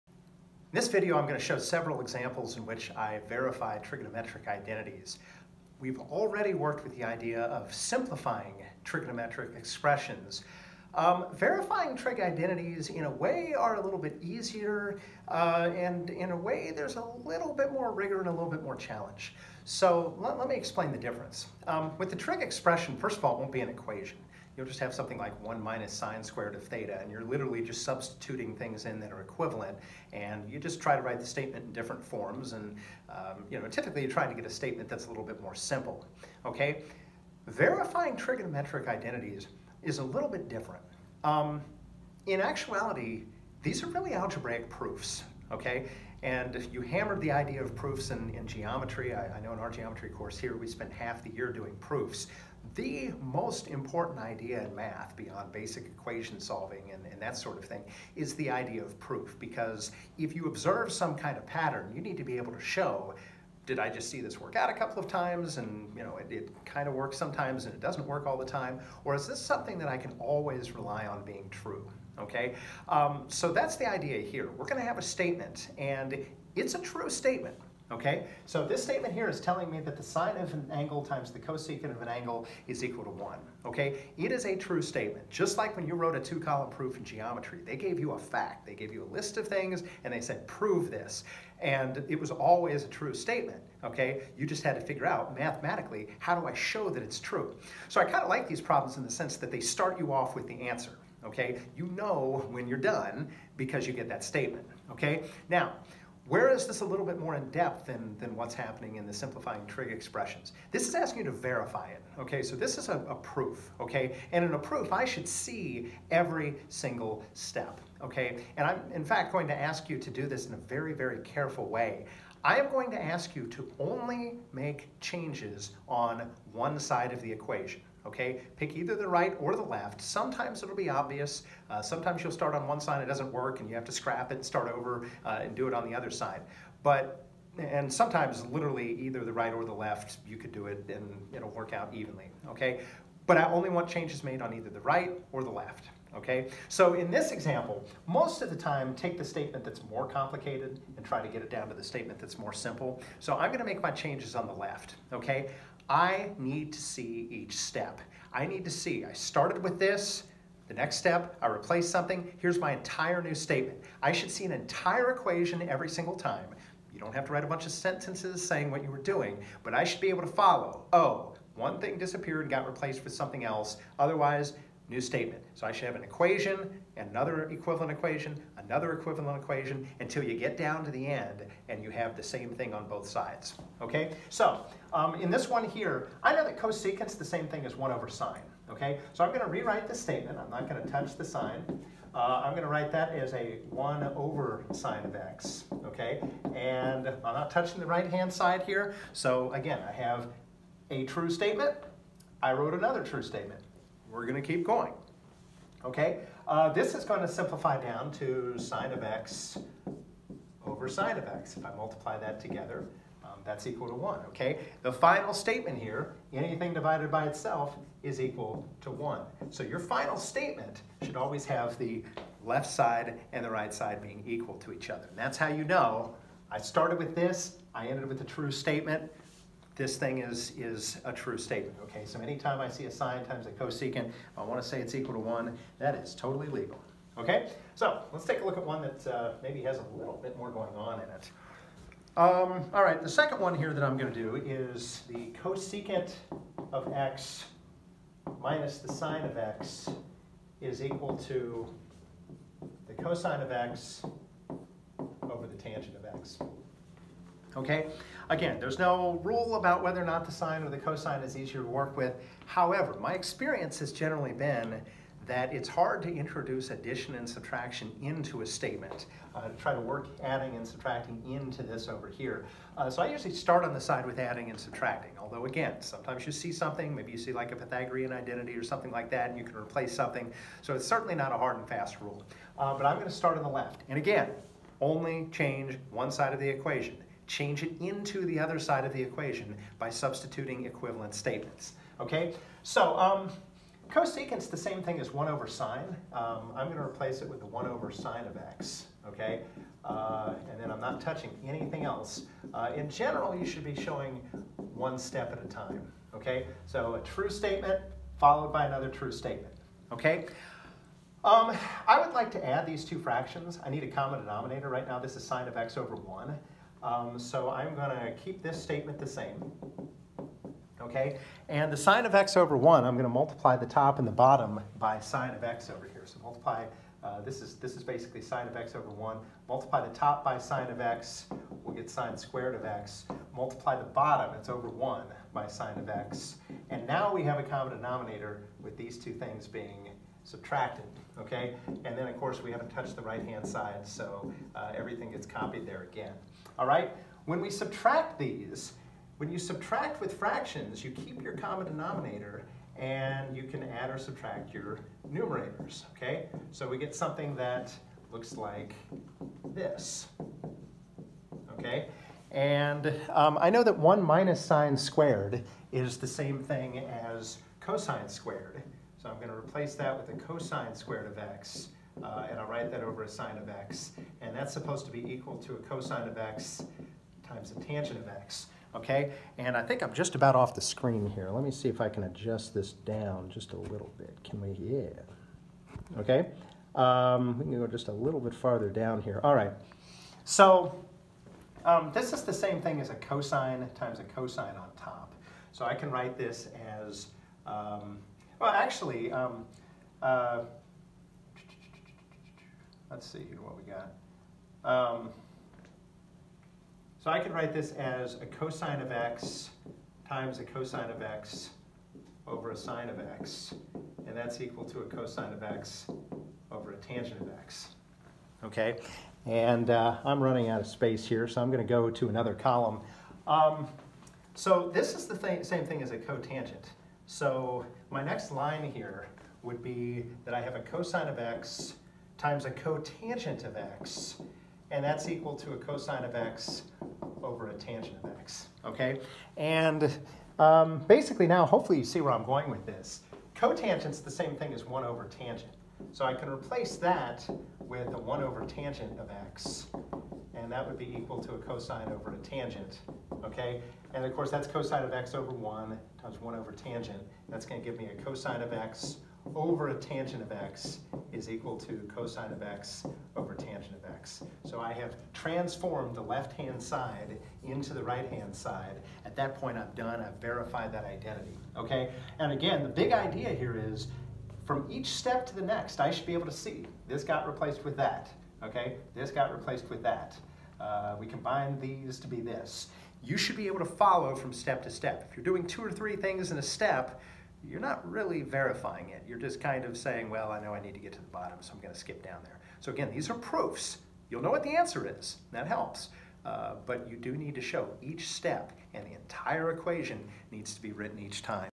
Intro to Verifying Trig Identities
Introduction to writing algebraic proofs to verify trig identities.